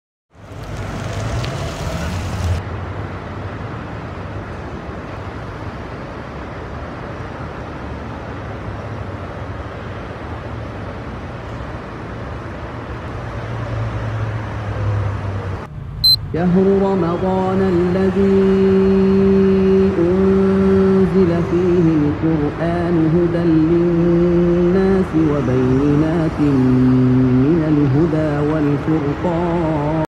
Ramazani është muaji i Kur'anit
Allahu (xh.sh) thotë në Kuranin famëlartë: “Muaji i Ramazanit në të cilin zbriti Kur'ani..”